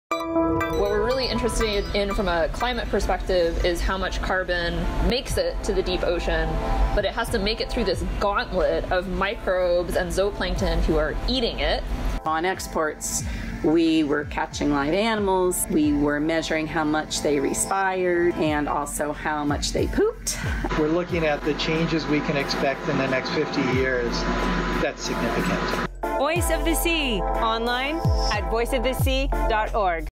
EXPORTS 3: Tracing Carbon Flow to the Deep Sea - VOS 7-7 (trailer)
In this episode of Voice of the Sea, we’re analyzing data from the EXPORTS research project with researchers from the University of Hawaiʻi, the University of Miami, the Virginia Institute of Marine Science, Woods Hole Oceanographic Institution, and the University of California Santa Barbara.

EXPORTS is a joint investigation, by NASA and the National Science Foundation, to study the ocean’s carbon cycle. During the summer of 2018, researchers headed out to the northeastern Pacific Ocean on board the Research Vessels Roger Revelle and Sally Ride. The goal was to to collect data about water chemistry, currents, and organisms —in order to better understand the ocean’s food web and how carbon-containing material makes its way from the surface to the deep sea. Since returning from the expedition, EXPORTS scientists have been identifying organisms, analyzing samples, and working together to make mathematical models of carbon flow in the ocean. The goal is to use these models, in combination with satellite imagery, to make real-time observations and predictions of the global climate from space. All EXPORTS data will be made publicly available online.

PC: Cover photo of a salp by NOAA Office of Ocean Exploration and Research, 2017 Laulima O Ka Moana

Learn more online at Voiceofthesea.org.